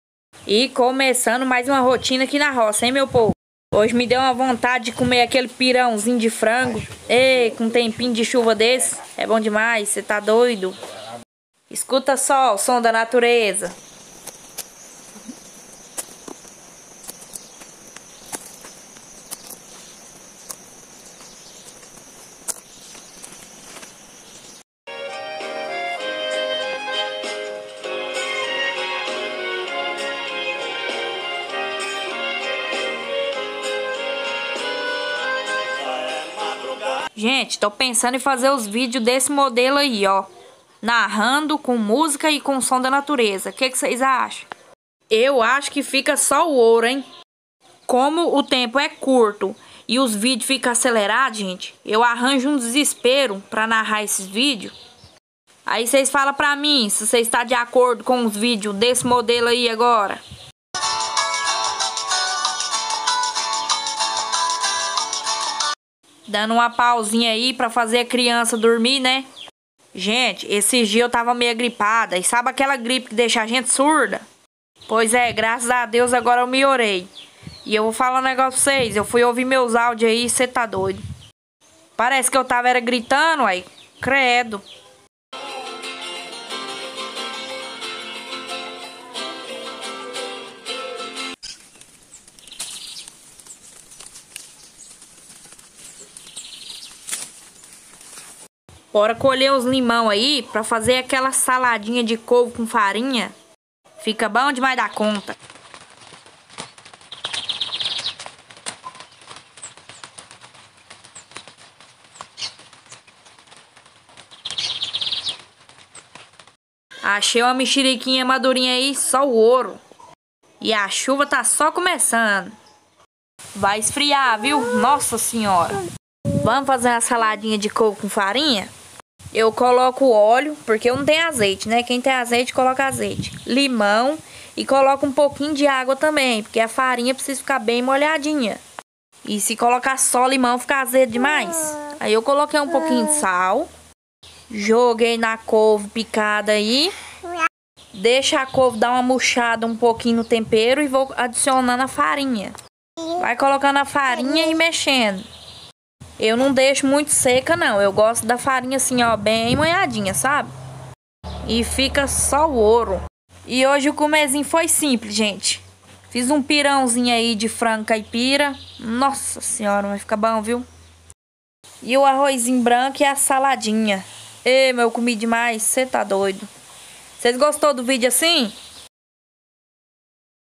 Hoje foi dia de fazer aquele delicioso pirão de frango! É bom demais cê tá doido 😃
Rotina, dicas, receitas e uma vida maravilhosa aqui na roça!